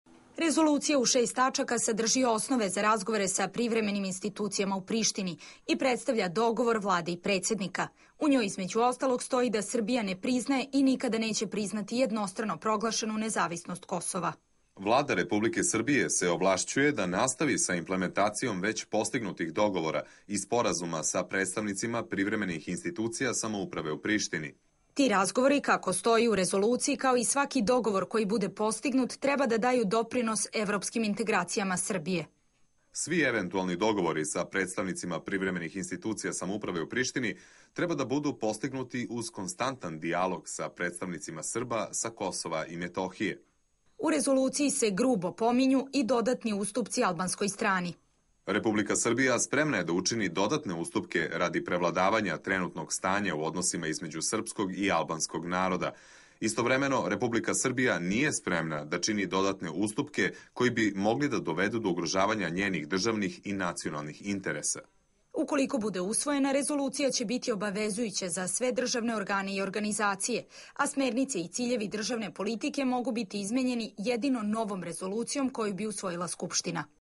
Predlog nove rezolucije o Kosovu
Na zajedničkoj sednici Vlade i predsednika Srbije usaglašen je tekst
predloga Rezolucije o Kosovu i Metohiji, o kojoj bi do kraja nedelje trebalo
da se izjasne i poslanici u Skupštini. Platforma je i dalje tajna, ali je
tekst rezolucije objavljen na sajtu Vlade i poslat Skupštini. Cilj ove
rezolucije je postizanje sveobuhvatnog rešenja za Kosovo i Metohiju.